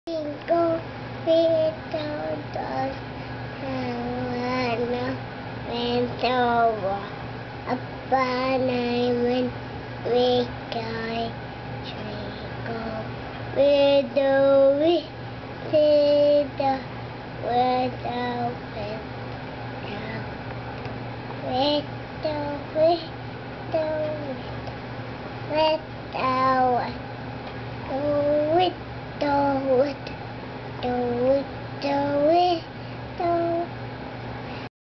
Name that tune 1
Mark will randomly break out in song. We love to sing with him. Although it is very hard to get on video. This morning we got two. Can you guess this one?